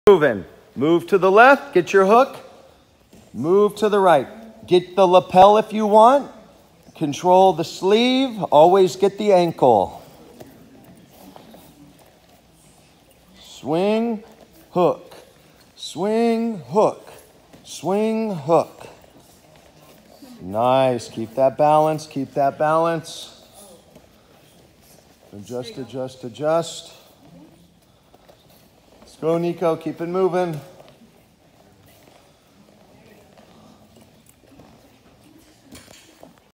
Kids BJJ WORKING
Kids BJJ team at Sentosa working getting their De La Riva Hooks